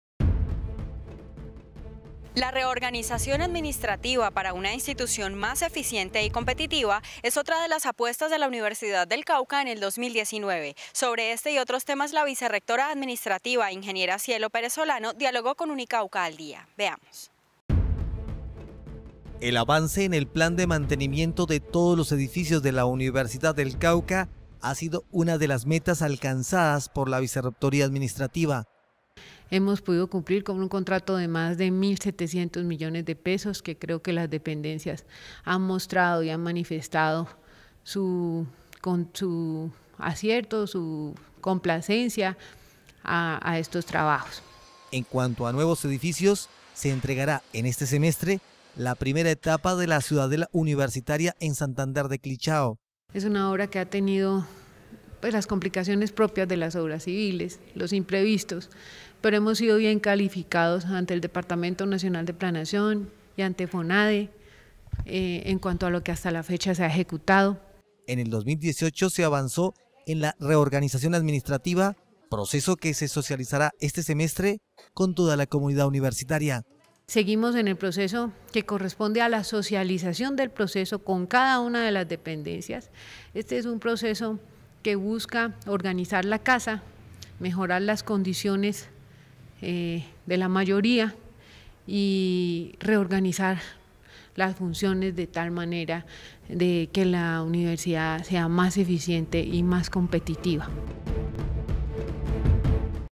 Balance 2018 y Prospectivas 2019. Vicerrectoría Administrativa de Unicauca
Cambios para mejorar procedimientos administrativos y avances en obras de la Universidad del Cauca, son algunos de los temas que abordó la Vicerrectora Administrativa, Cielo Pérez Solano, durante su informe de logros 2018 y desafíos de esta dependencia universitaria para el 2019.